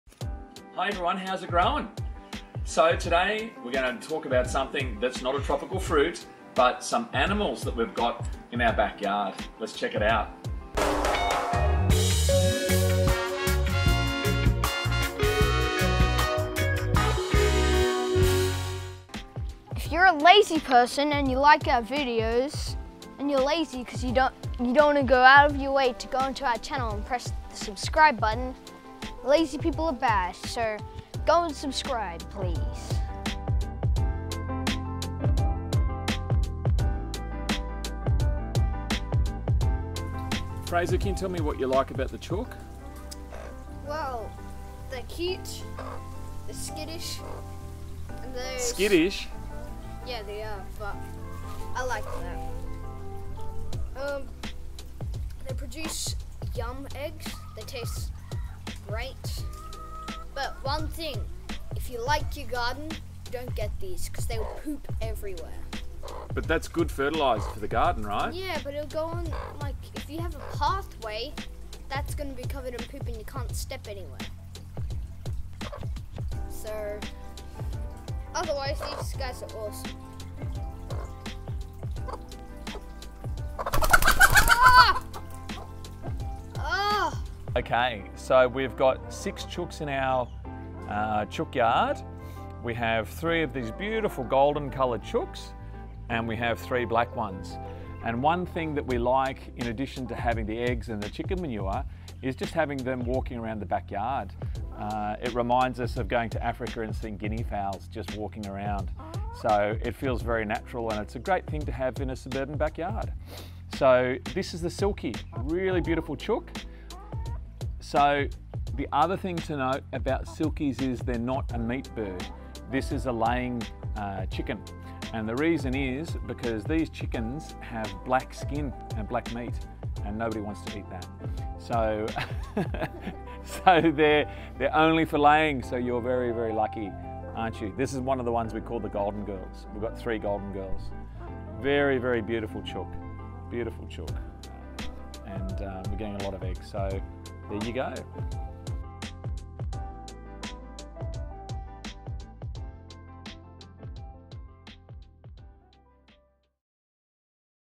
Silkie Chickens
A quick overview of our Silkie chickens. A great breed for a small backyard.